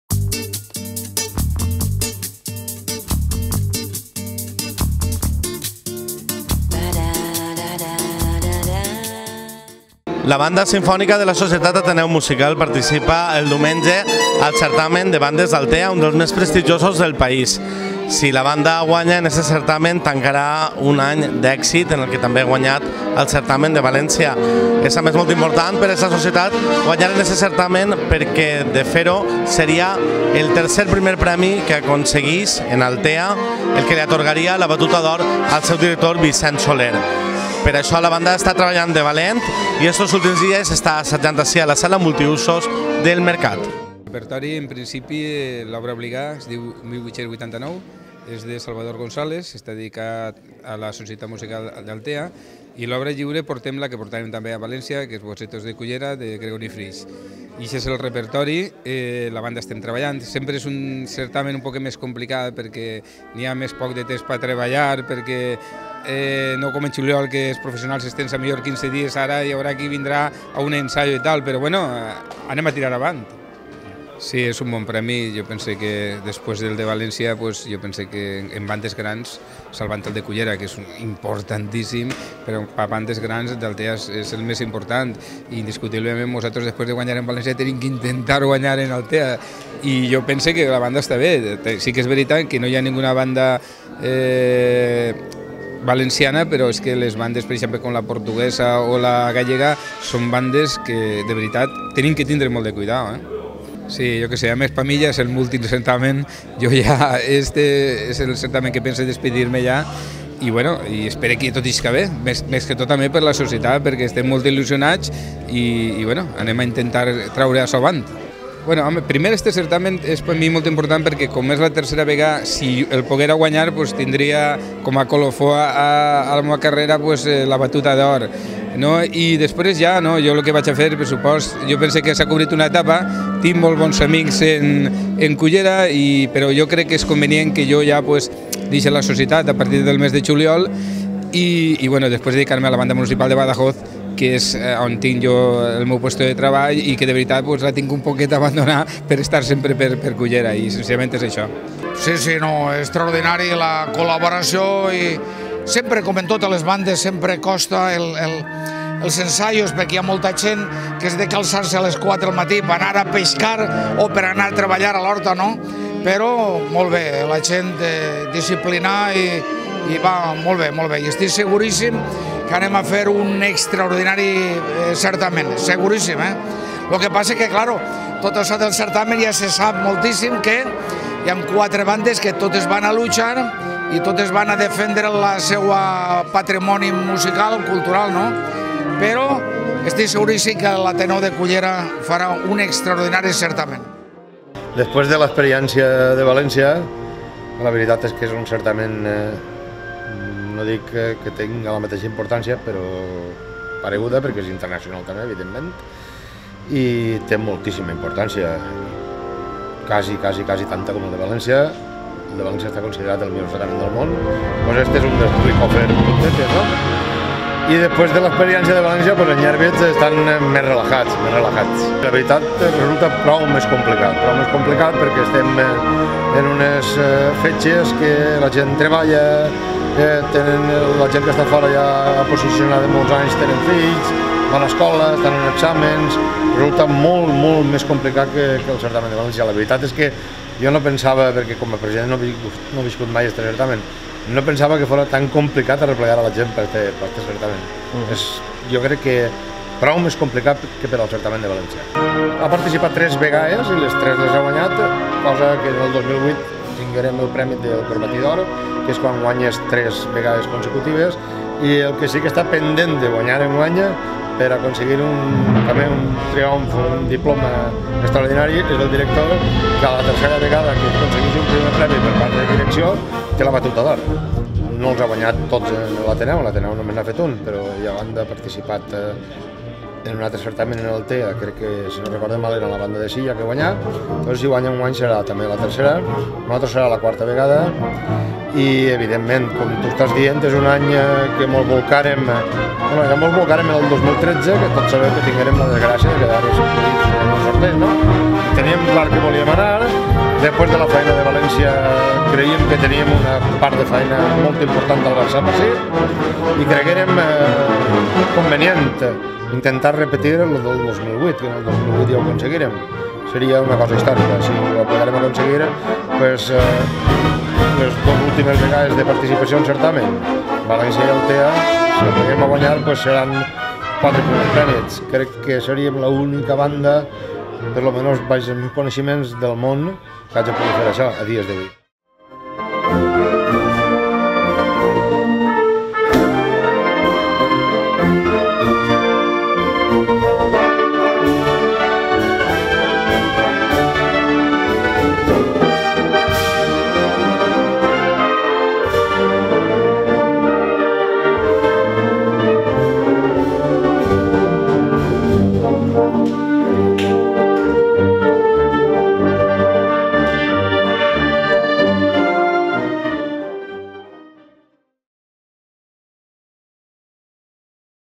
L'ATENEU MUSICAL ES PREPARA PER AL CERTAMEN VILA D'ALTEA
VÍDEO: la banda simfònica de l'Ateneu Musical continua els assajos al multiusos del Mercat per al Certamen internacional de bandes Vila d'Altea que es celebrarà el proper diumenge 7 de desembre. Més info